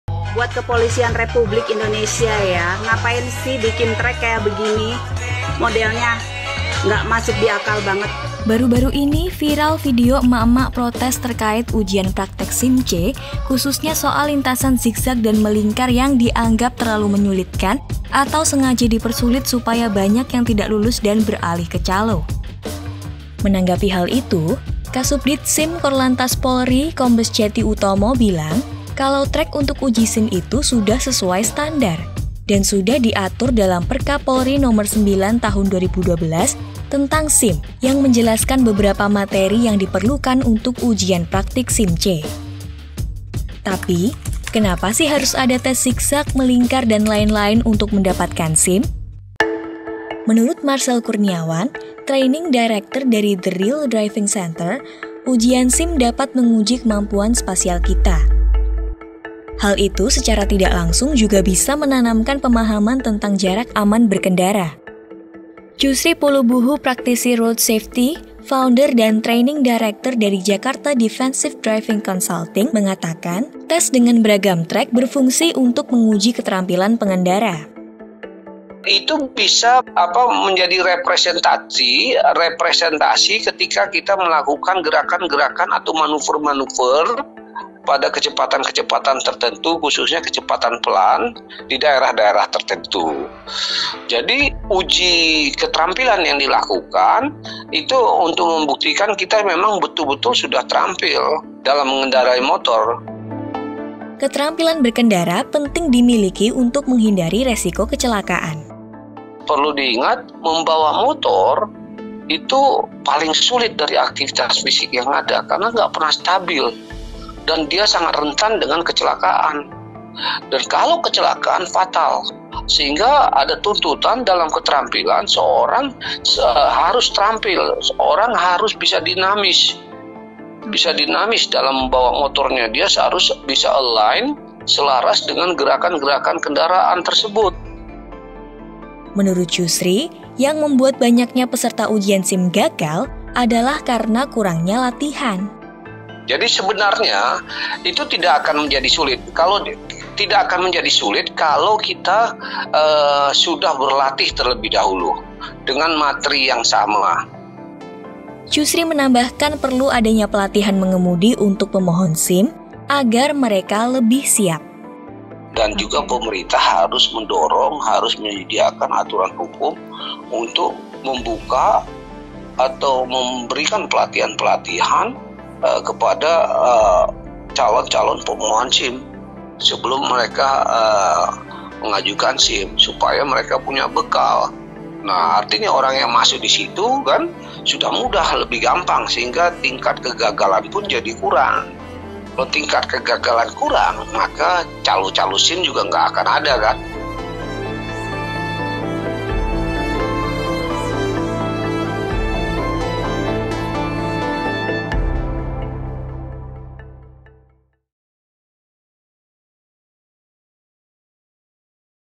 Emak-emak Heboh Protes Ujian SIM. Kenapa Harus Ada Trek Zig-Zag & Angka 8? | Tech It Easy
Beberapa waktu lalu sempat ramai jadi perbincangan, video seorang emak-emak yang protes tentang trek ujian praktik SIM C yang melingkar dan zig-zag itu.

Katanya, itu tidak masuk akal dan sengaja dipersulit.

Tapi sebenarnya kenapa, sih, kok treknya dibuat serumit itu?

Simak penjelasannya di video ini, ya~

(Narasi)